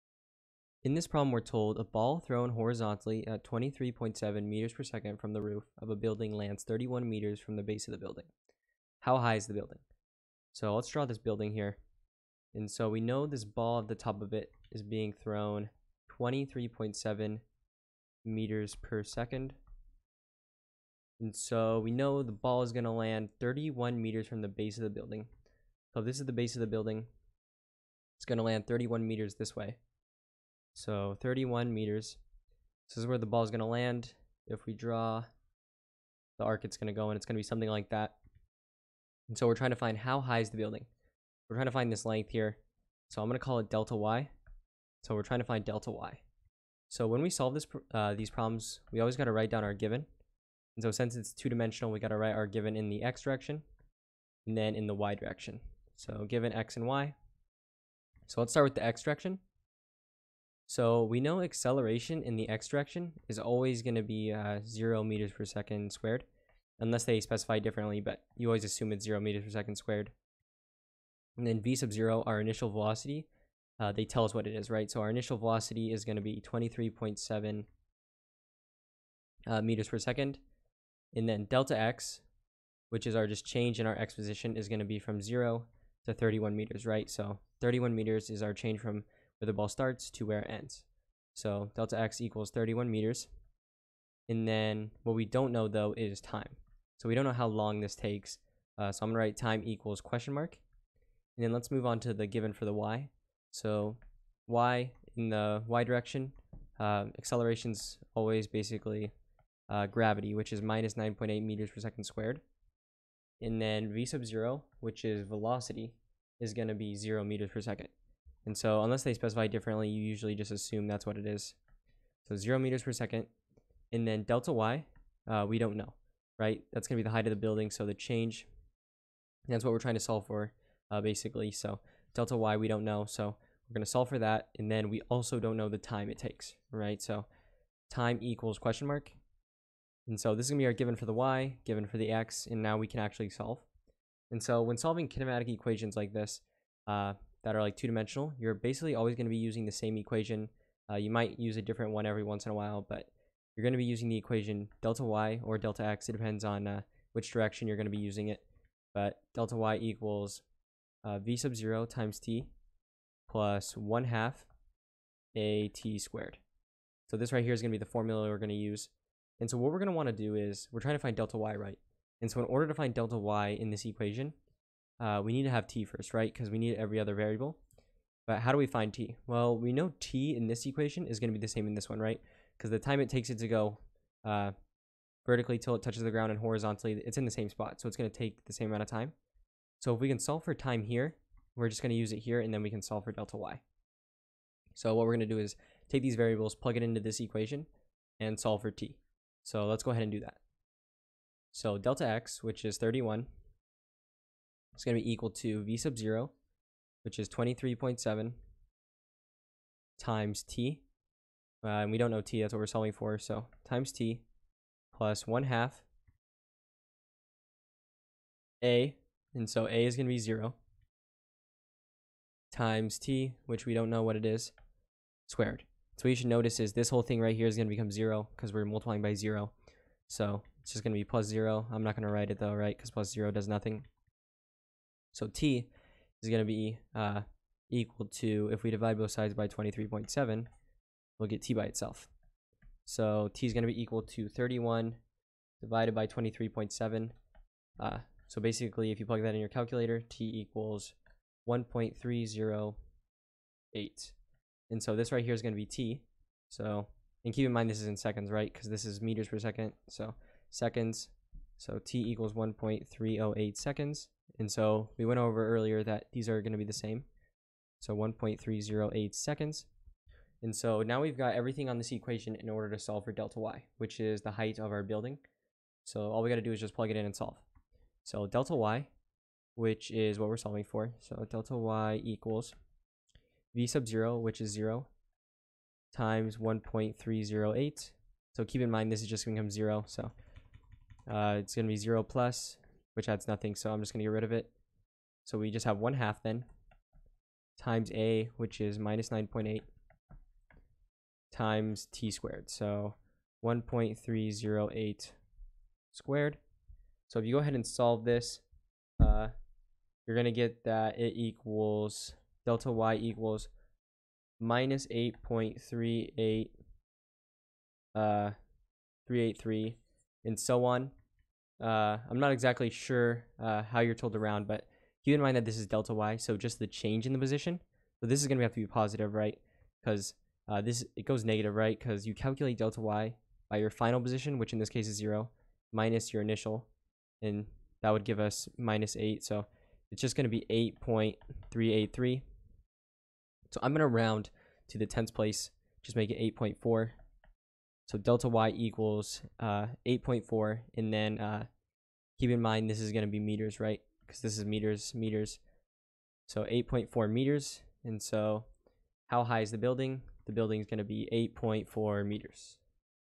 A ball thrown horizontally at 23.7 m/s from the roof of a building lands 31.0 m from the base.
34. (II) A ball thrown horizontally at 23.7 m/s from the roof of a building lands 31.0 m from the base of the building. How high is the building? .